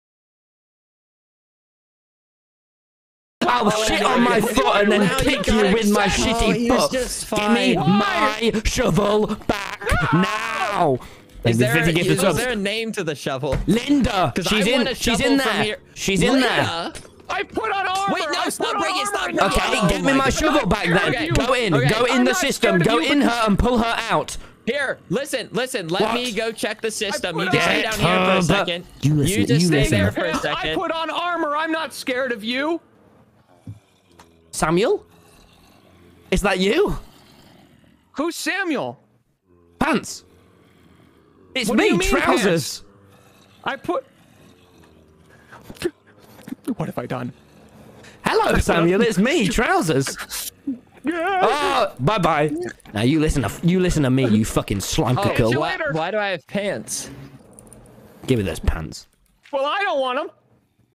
TommyInnit at Work
Tommy flips like a switch during rp and it's amazing.
My appreciation for Tommy's roleplay & improv grows every time he performs.